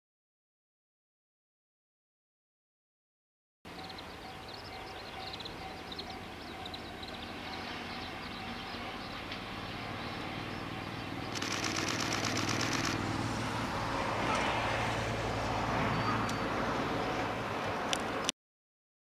EF64 1030＋12系高崎車4B 「DL青い12系客車」送り込み回送(回9737レ)@2019.6.13
6月13日、6月15・16・22・23日に運転される"DL青い12系客車"に使用するための高崎車両センター高崎支所所属の12系4両が高崎～新津間で回送されました。

牽引は長岡車両センター所属のEF64 1030でした。